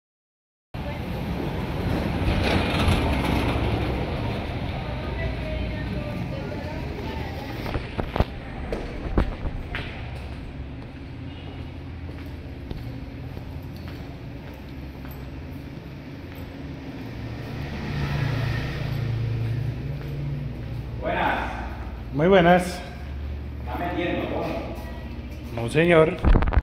LOCAL EN ARRIENDO EN SALITRE
Local en arriendo ubicado en salitre cuenta con 100 mts2, dos baños privados, sus pisos son en cerámica tiene buena iluminación natural y amplios espacios para uso comercial, a poca distancia de transporte público y zonas verdes. cuenta con vías de acceso por la calle 26, y Cra 68. para más información comuníquese con el ejecutivo comercial Yan Fabio Quiroga al celular o WhatsApp , al correo yan.quirogainmobiliariasaliadas.com y dejar sus datos en contactar al anunciante.